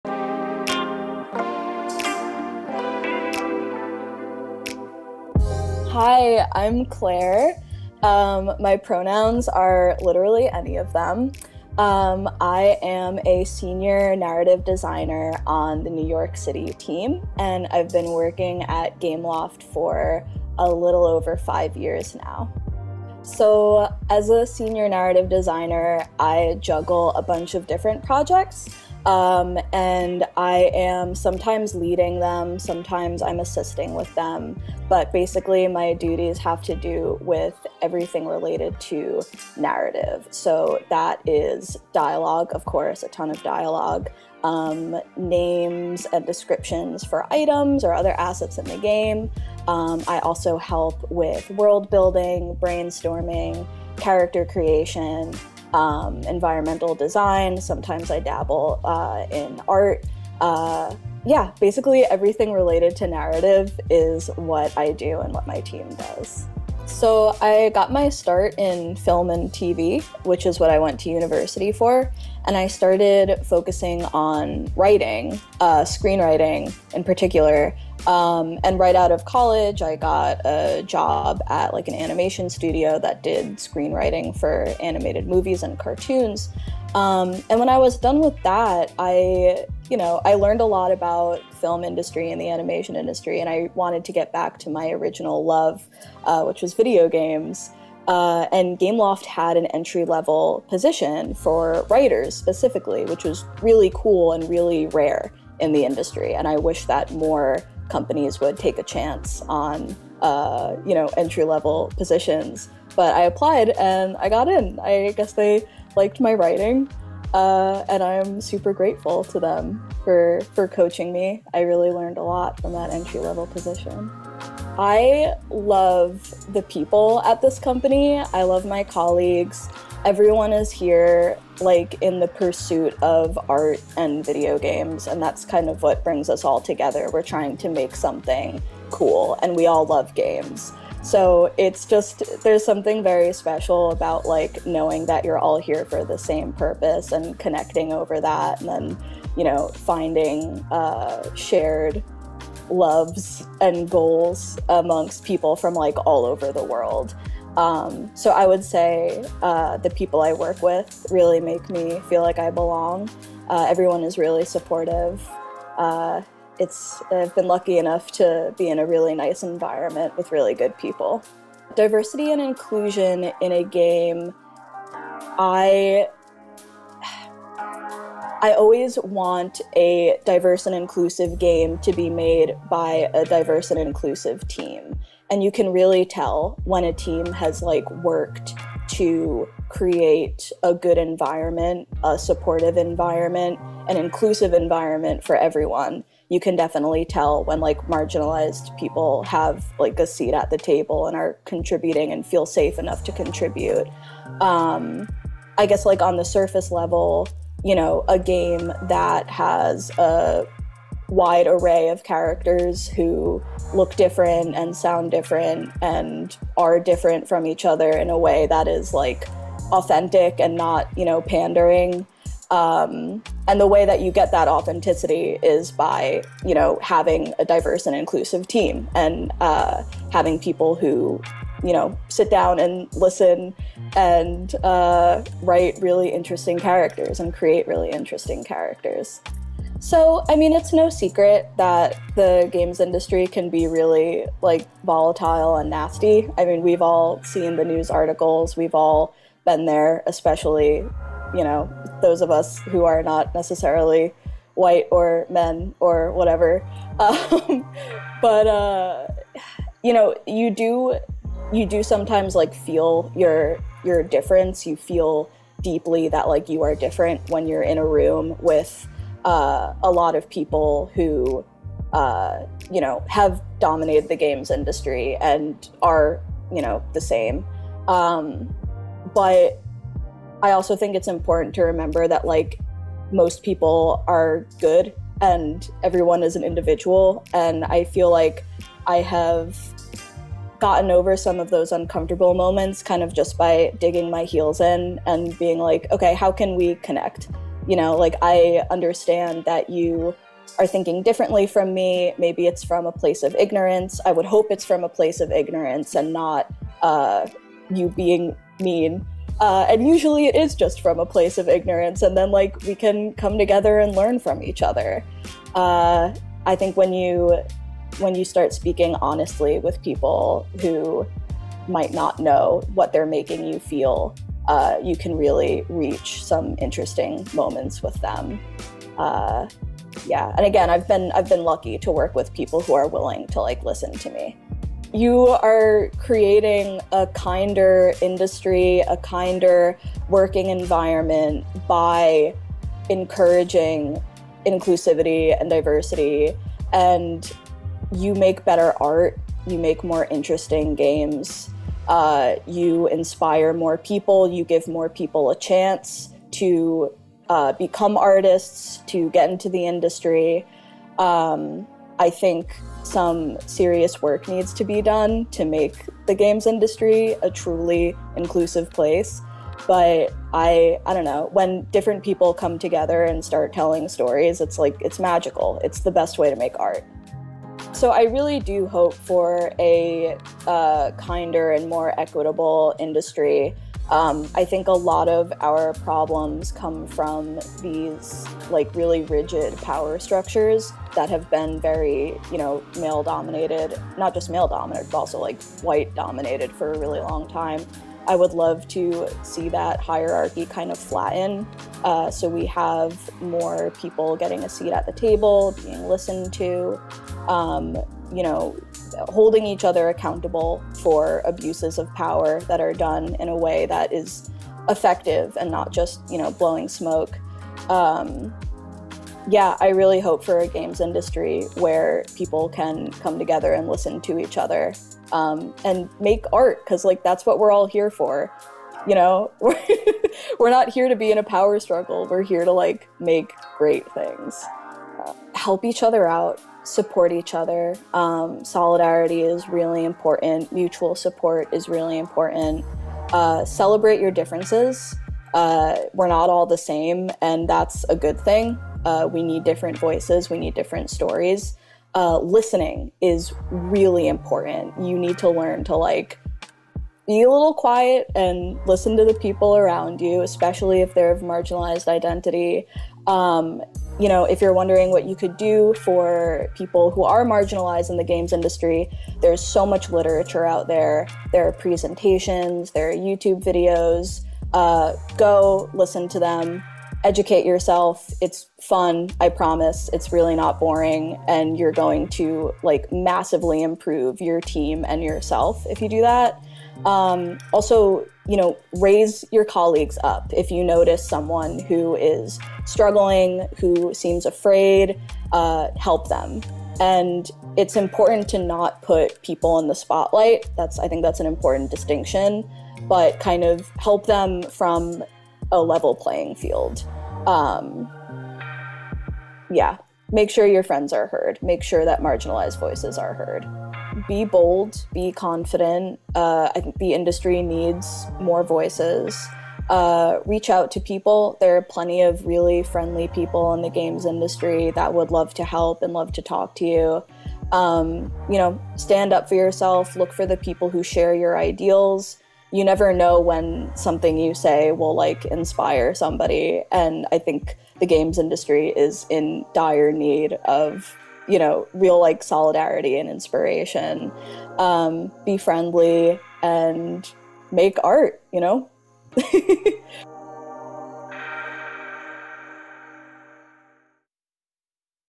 We Belong Here - Claire Stenger
We're closing out our interviews with Claire Stenger, Senior Narrative Designer at Gameloft New York.  In her eyes, "you are creating a kinder industry, a kinder working environment by encouraging inclusivity and diversity."

Find out what diversity and inclusion in a game look like to her and why she feels like she belongs in the gaming industry!​